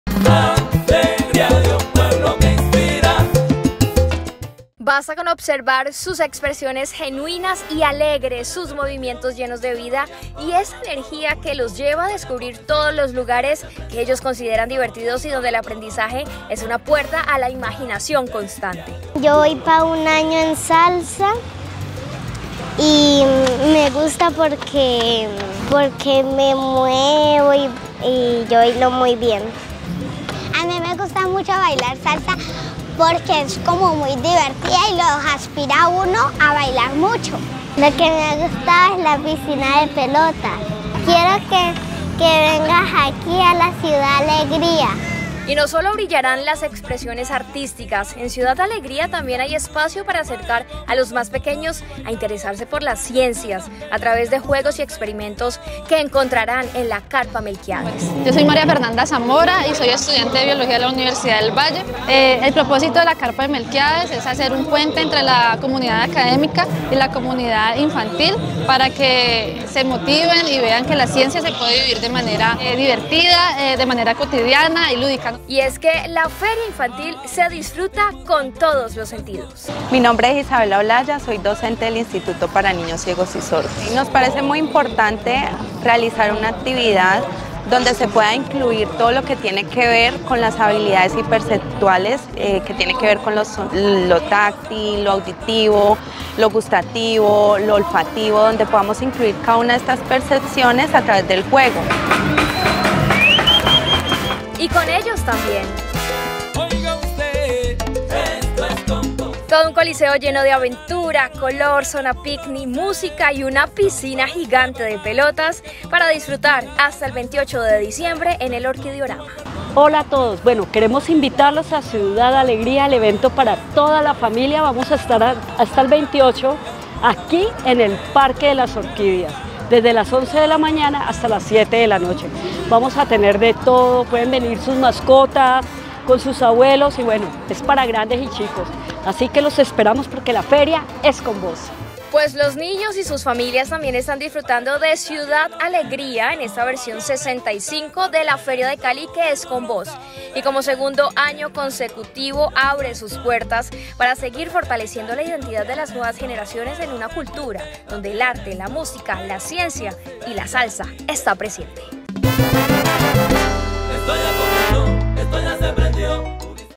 ¡CIUDAD ALEGRÍA, LA FERIA PARA TODA LA FAMILIA!
Ciudad Alegría es el evento de la FeriaDeCali2022 para que disfruten grandes y chicos. Juegos, música, baile, ciencia, espacio para las mascotas y mucho más.

📍Parque de las Orquídeas - Av 2N # 47-02
🕚11AM - 7PM
🎟ENTRADA LIBRE

Te esperamos a ti tu familia porque